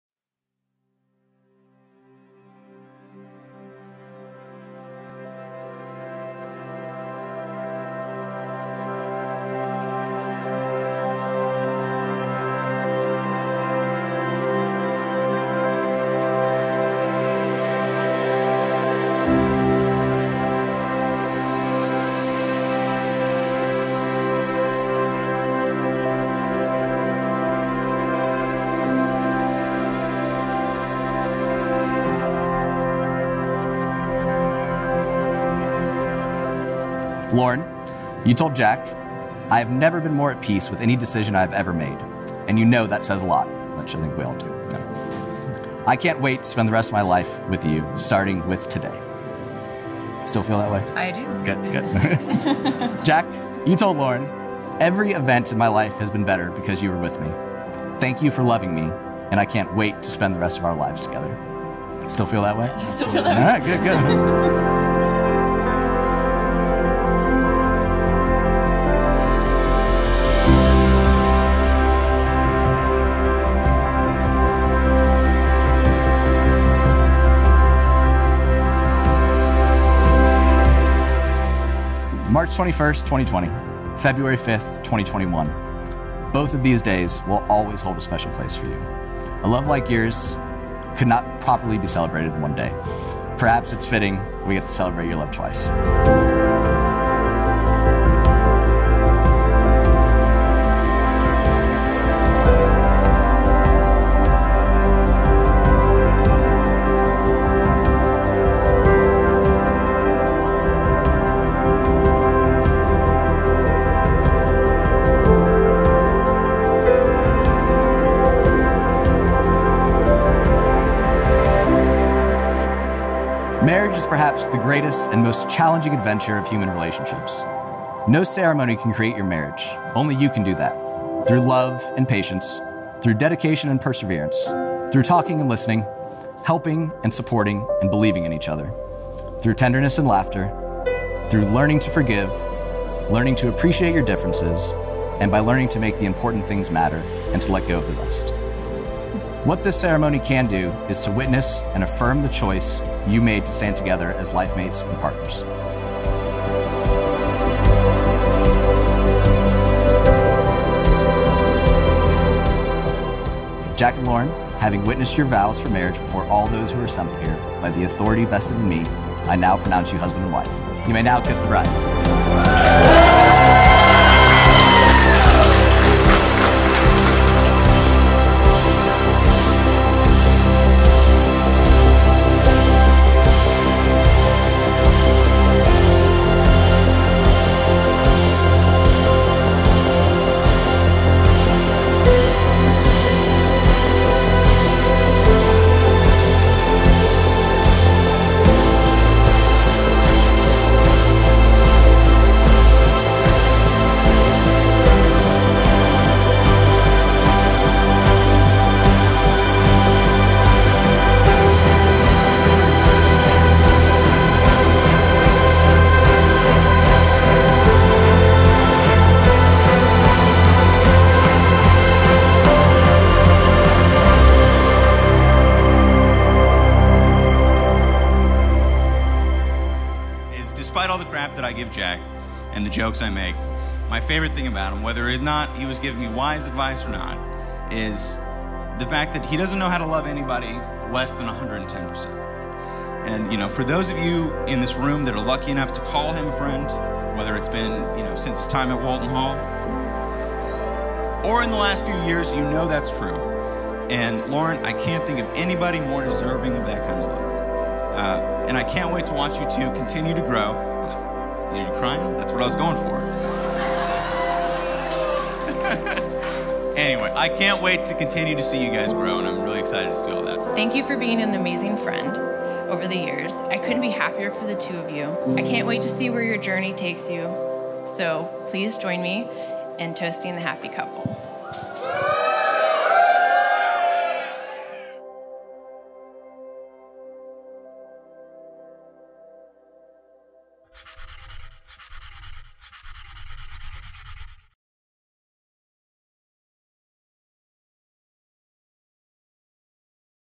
A Rustic Fairytale Wedding at BRIK Special Events Venue
Lauren and Jack’s rustic fairytale is chock full of white anemones, neutral accents, and a whole lot of love for the couple! While husband and wife duo Laning Photography captured all the moody and rustic details of the day, Silver Bear Creative captured Jack and Lauren’s wedding in a different view with their modern videography skills seen here. We couldn’t be more excited for the new Mr. and Mrs. Fitzgerald! Read their sweet story and find their full photo gallery here. Cheers!